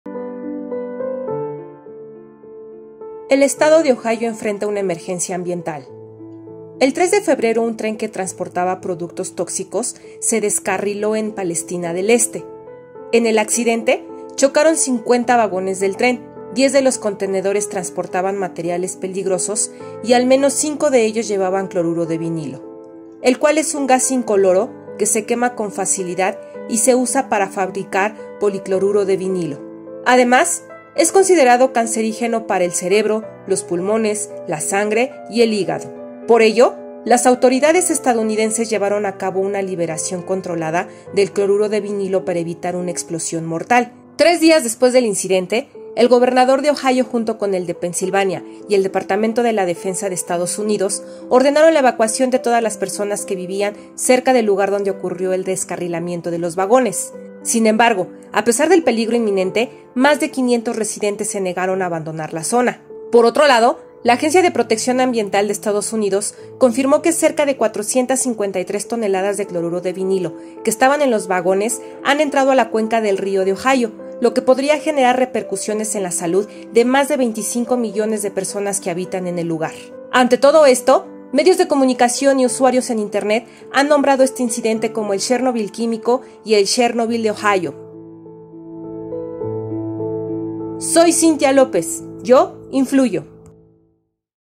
El estado de Ohio enfrenta una emergencia ambiental
El estado de Ohio se convirtió en el escenario de la explosión de cloruro de vinilo, que ha causado alerta no solo en EUA, sino en todo el mundo.